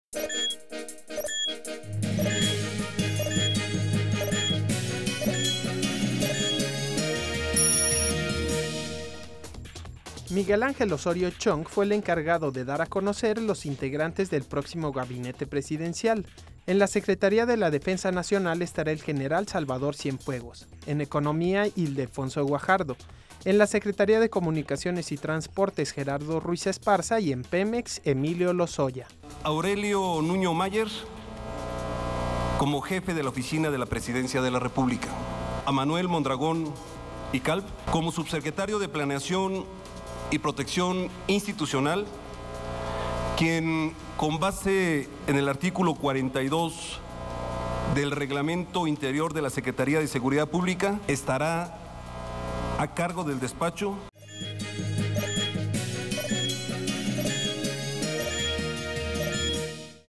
Corte 44 / Miguel Ángel Osorio Chong presentó al Gabinete de Peña Nieto parte 2 / Nov 30 2012
Corte 44 -- Quien será Secretario de Gobernación en la Administración del Presidente Enrique Peña Nieto, presentó hoy a los integrantes del Nuevo Gabinete Presidencial.